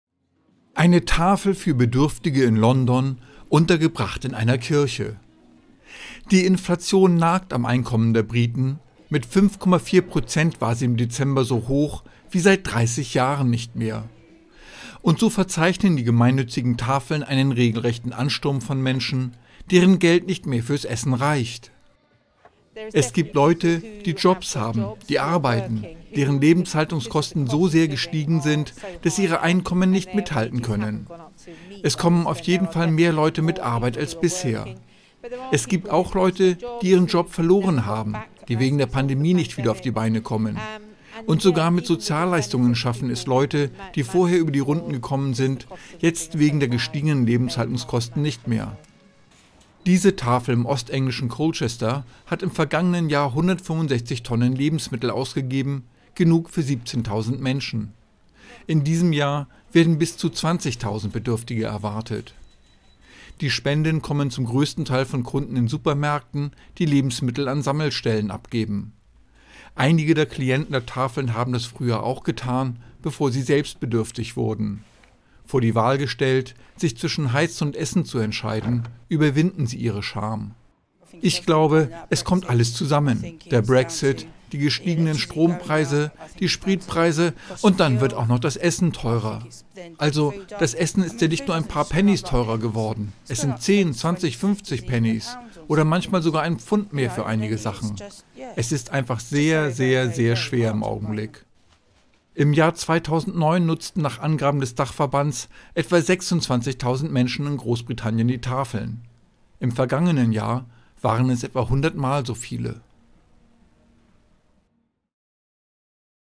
Nach dem Brexit: Inflation löst Ansturm auf britische Tafeln aus
In Großbritannien sinken die Reallöhne, während die Lebensmittel- und Energiekosten steigen. Die Einkommen vieler Briten reichen nicht mehr aus, um die Lebenshaltungskosten zu decken.